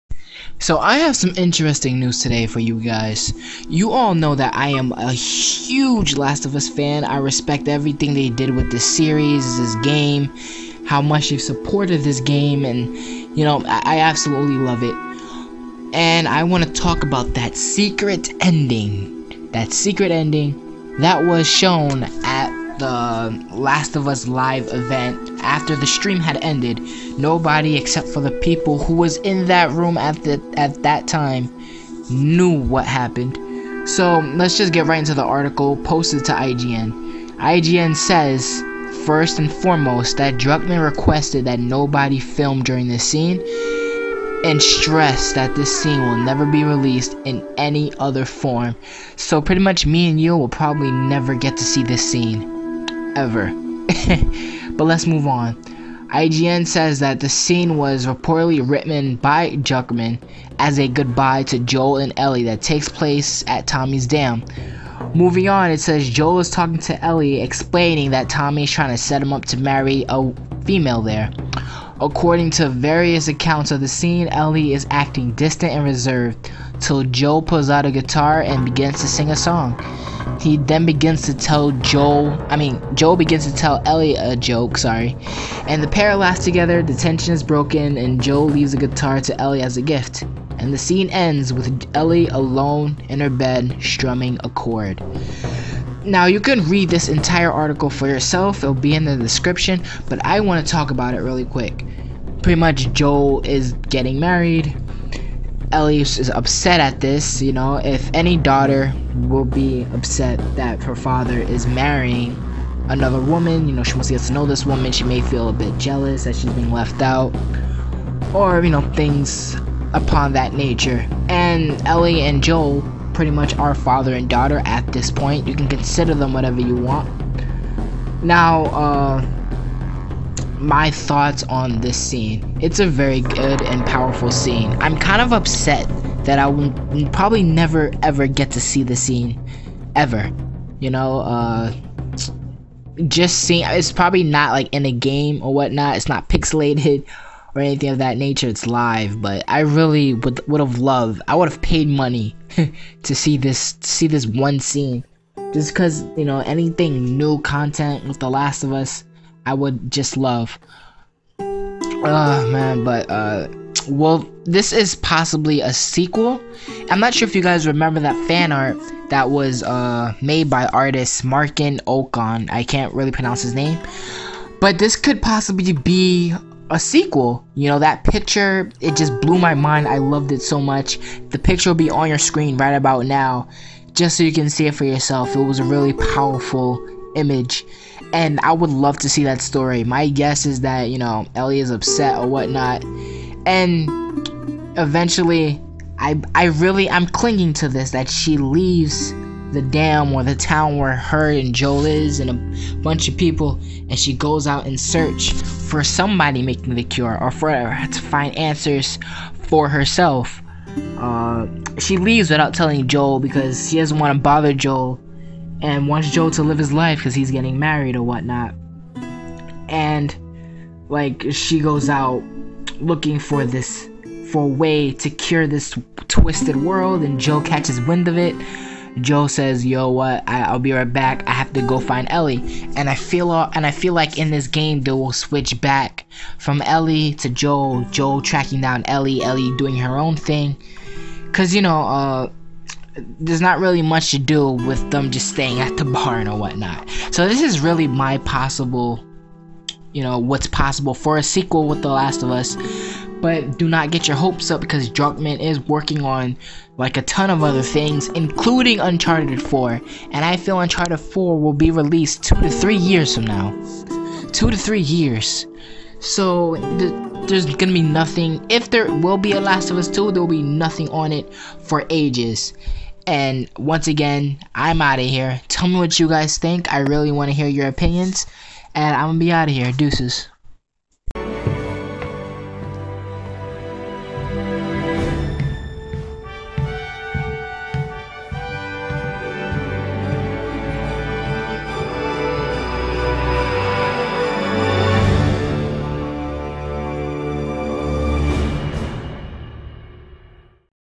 The Last of Us "Secret Ending" at live event disscussion
Naughty Dogs's The Last of Us had a live event on 7/28/14 which was FANTASTIC! But i'm mainly here to talk about the secret ending that performed off stream.